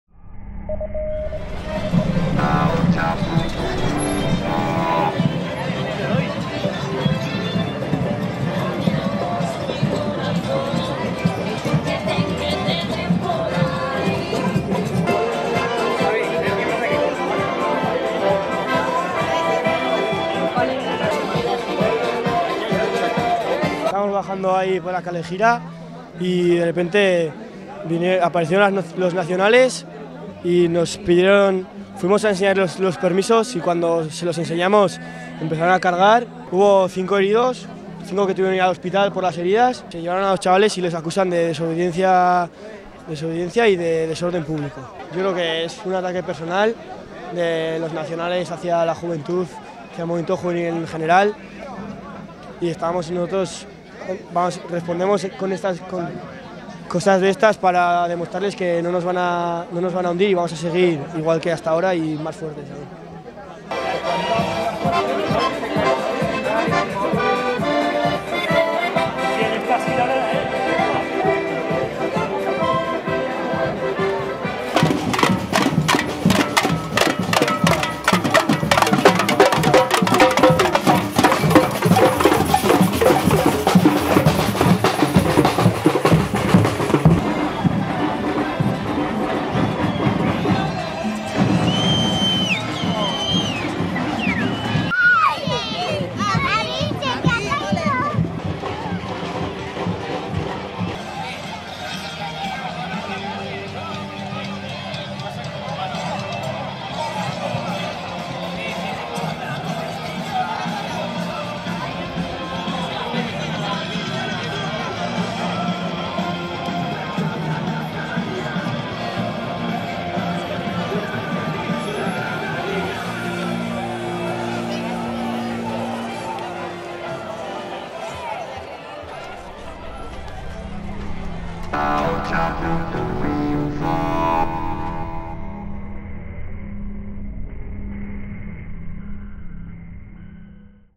Mendillorri contra la represión
Una kalejira con ambiente festivo y colorido ha recorrido las calles de Mendillorri para denunciar la carga policial que el pasado jueves dejó 5 heridos y las posteriores 2 detenciones que la Policía española efectuó esta semana.